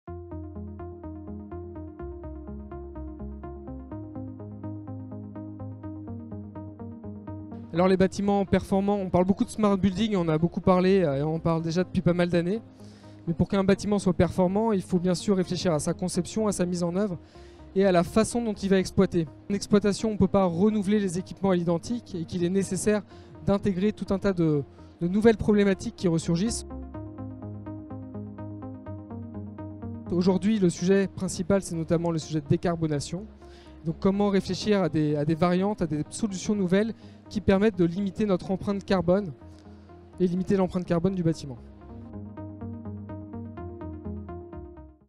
🎙Smart building : vers le bâtiment zéro énergie | Bouygues Energies & Services
🏢 Benjamin Rulens, Responsable performance énergétique et immobilier durable nous en parle.

💬 "Pour qu'un bâtiment soit performant il faut réfléchir à sa conception, à sa mise en oeuvre et à la manière dont on va l'exploiter. L'objectif principal est de limiter l'empreinte carbone des bâtiments."